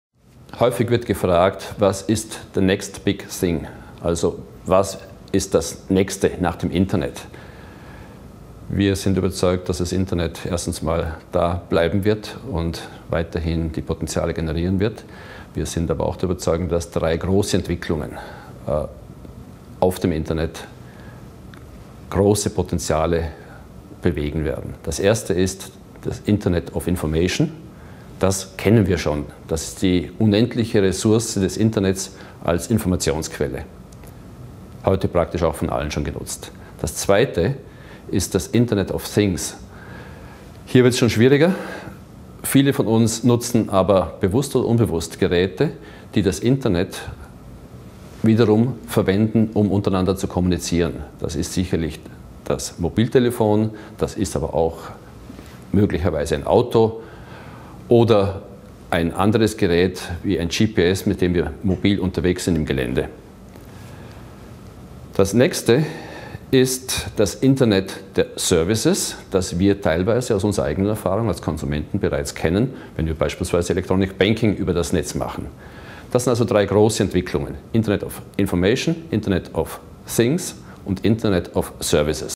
Prof. Dr. Hubert Österle - Was kommt nach dem Internet?
Es wird drei große Entwicklungen basierend auf dem Internet geben. Das Internet of Information, das Internet of  Things und das Internet of Services.